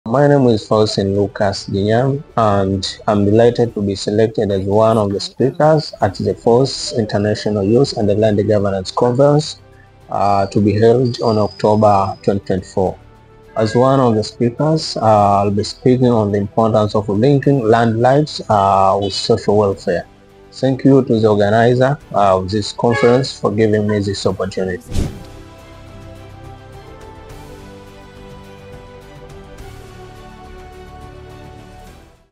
Preview of the Keynote Speech of Faustine Lucas Jinyami at CIGOFA 4
Faustine Lucas Jinyami is a dedicated advocate with a Bachelor's degree in Social Work from the University of Dar es Salaam in Tanzania. He is a committed advocate for the land rights of women and young girls.

He passionately champions equal access to land ownership, focusing on empowering women and girls both economically and socially, particularly in rural and marginalized areas. Faustine is devoted to raising awareness and driving legal reforms to ensure that women and girls can fully realize their land rights.